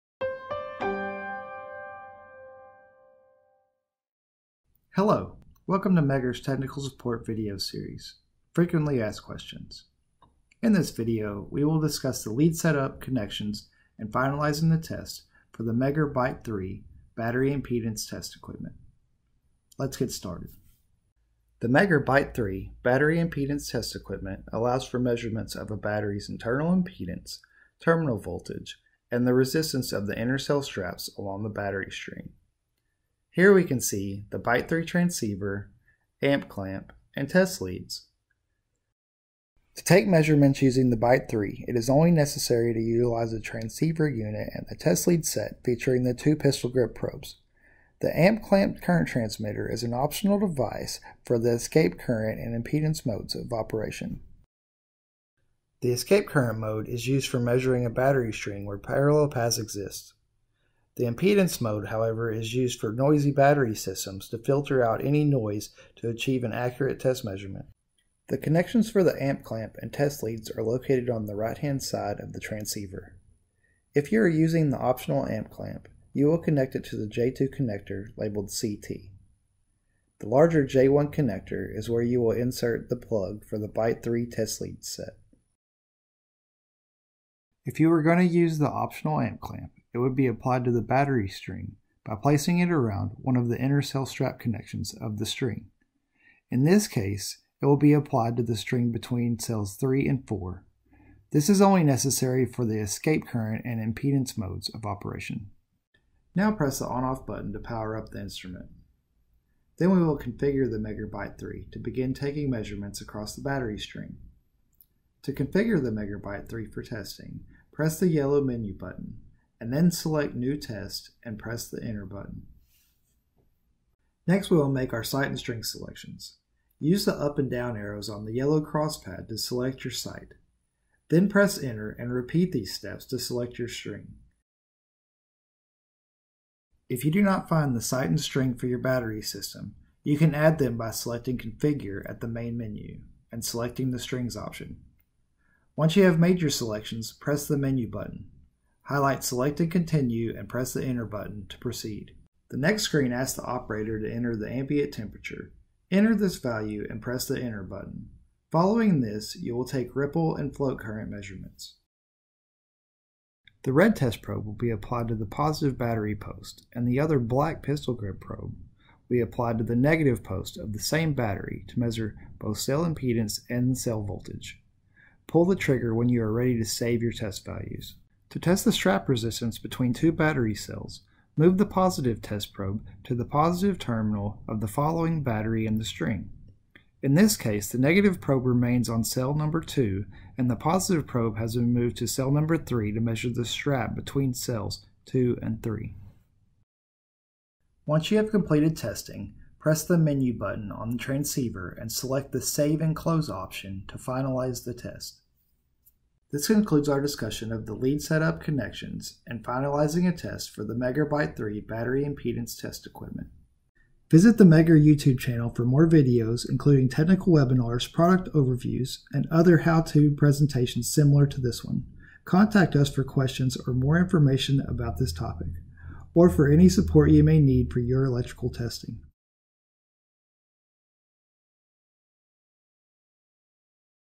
Megger BITE3: How to Perform Battery Ohmic Testing
In this video we will discuss the Megger BITE 3 Battery Impedance Test Equipment procedures for battery ohmic testing.  This will include connecting the amp clamp and test leads to the transceiver, operation of the transceiver instrument, application of the test leads to the battery string, and finalizing the test upon completion.  The BITE 3 is used for taking battery impedance, cell voltage and intercell strap resistance measurements.

The Megger BITE3 Battery Impedance Test Equipment determines the health of lead-acid cells by taking measurements of the most important battery parameters. The BITE3 measures cell impedance, an internal ohmic test, cell voltage, intercell connection resistance and ripple current. And, for the first time in a battery instrument, the BITE3 measures float current and the harmonic content of the ripple current. There is even a built-in spectrum analyzer to show the harmonic content of the ripple current.